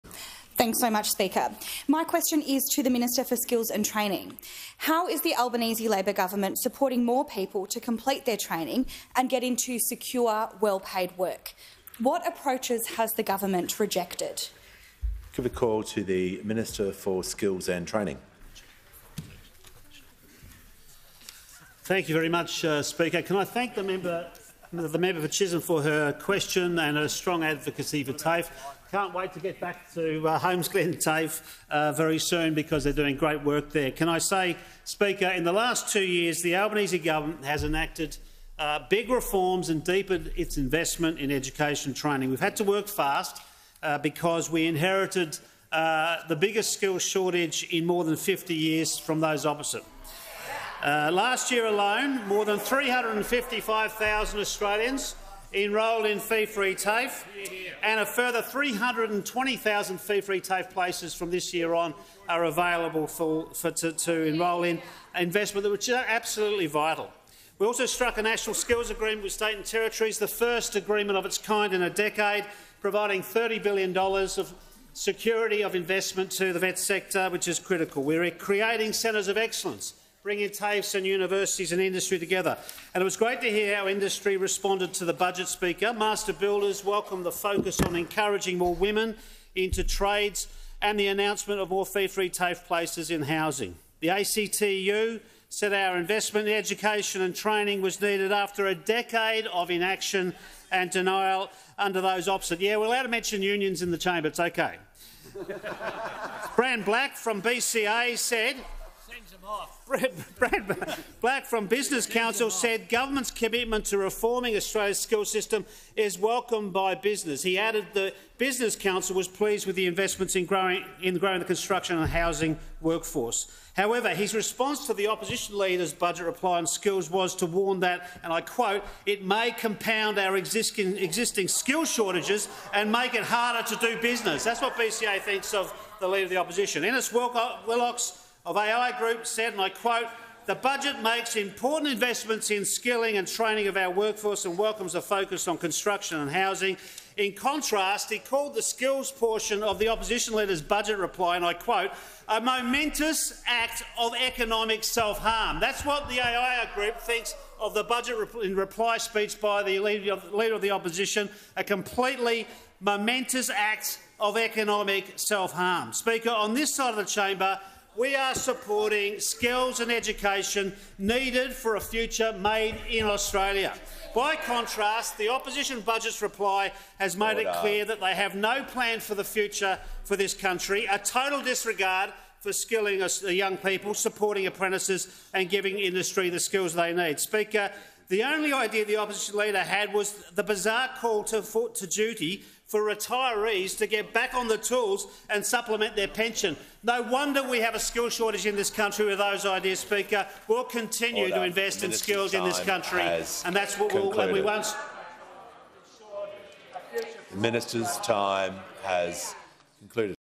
Labor govt has enacted 'big reforms' and made 'deep investments' in education training
Minister for Skills and Training Brendan O’Connor says the Albanese government has “enacted big reforms” and “deepened its investment” in education training.

“We inherited the biggest skill shortage in more than 50 years from those opposite,” Mr O’Connor said during Tuesday’s Question Time.

“Last year alone, more than 355,000 Australians enrolled in fee-free TAFE and a further 320,000 fee-free TAFE places from this year on are available to enrol in.”